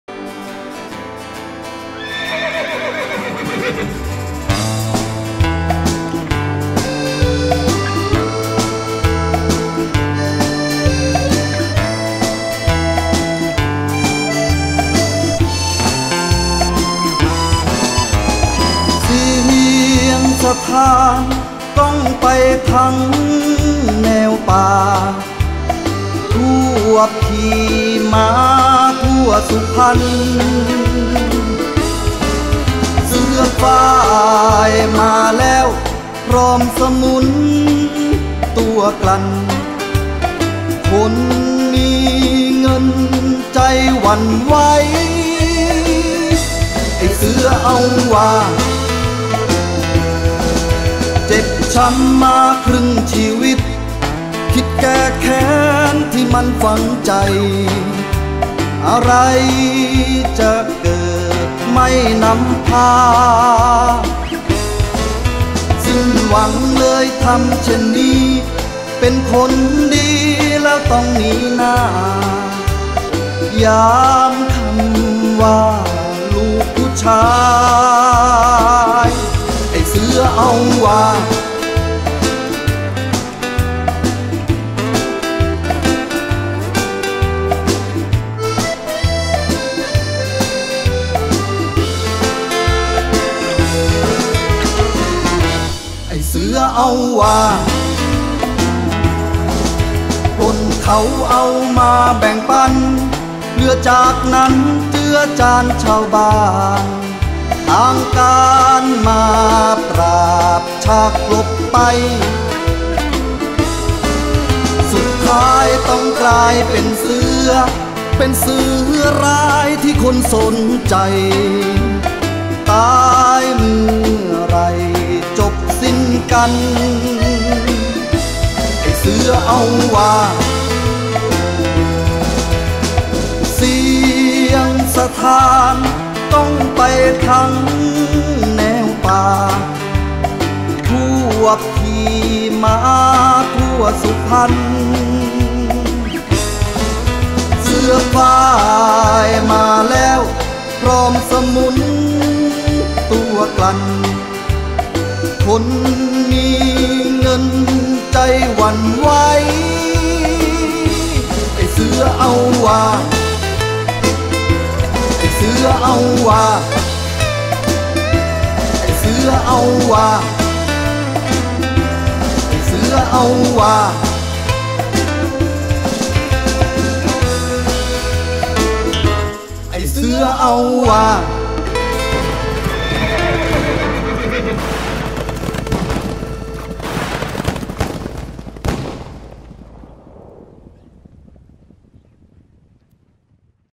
งานย้อนวันวานเมืองสุพรรณ เสือร้ายตำนานนอกกฎหมายเมืองสุพรรณ
วันที่ 3-5 ธันวาคม 2561 ณ สนามหน้าที่ว่าการอำเภอ

เดิมบางนางบวช  จังหวัดสุพรรณบุรี